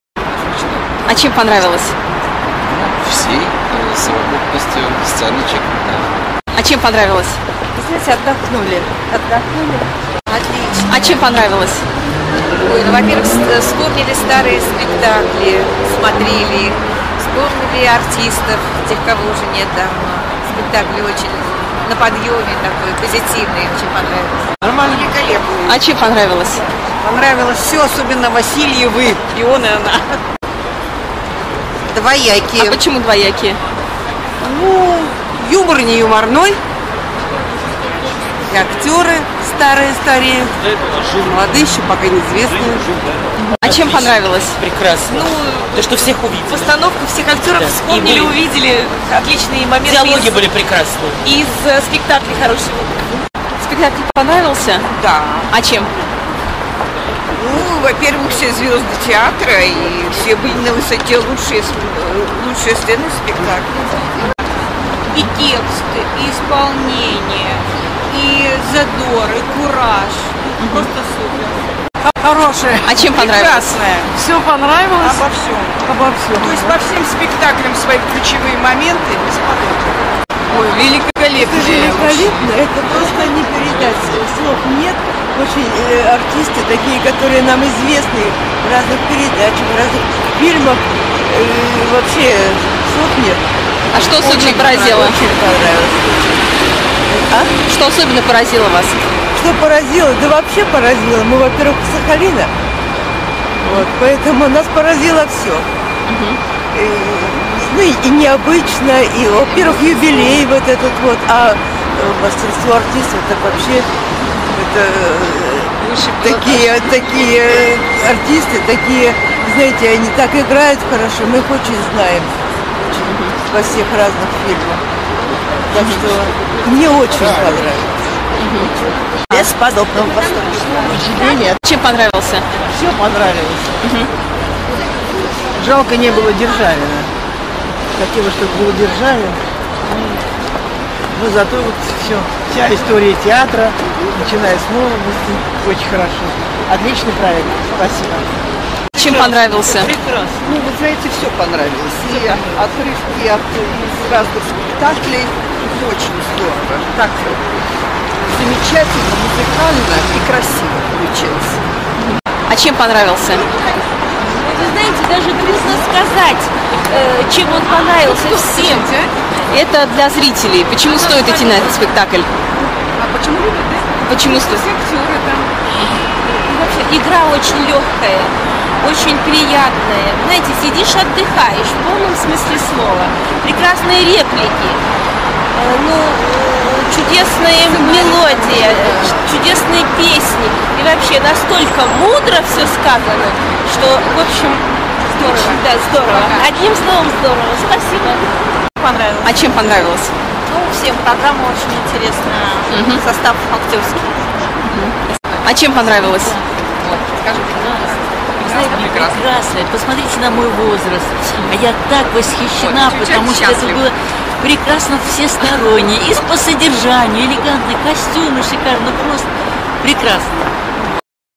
Грустно, но смешно отзывы, Театр Сатиры 7.10.2014
Отзывы зрителей о спектакле "Грустно, но смешно" в Театре Сатиры 7 октября 2014 года

Грустно, но смешно, Театр Сатиры

Театр Сатиры предлагает публике спектакль, котором занято немало прославленных звезд. В нем представлен целый коллаж из старых добрых номеров в исполнении как известных, так и молодых актеров театра. Отзывы публики на эту постановку в большинстве очень доброжелательные. Она создана в традиционном жанре капустника, поэтому в ней очень много по-настоящему забавных моментов. Особенно радуют зрителей старшего поколения номера с участием прославленных актеров: очаровательных Веры Васильевой и Натальи Селезневой, Юрия Васильева и других. Блистали остроумием Федор Добронравов и мэтр театра Александр Ширвиндт. В спектакле очень много самых разнообразных музыкальных произведений, она создает приподнятое и радостное настроение. Хроника, которая показывается в спектакле, рассказывает о корифеях театра, тех, кто были первыми его актерами. Атмосфера в зале очень доброжелательна, а отзывы зрителей наполнены добрыми чувствами по отношению к старшему поколению актеров. Но во многих номерах чувствуется налет легкой грусти от того, что не увидеть уже на сцене многих блистательных артистов, покорявших публику своей великолепной игрой и непередаваемым обаянием.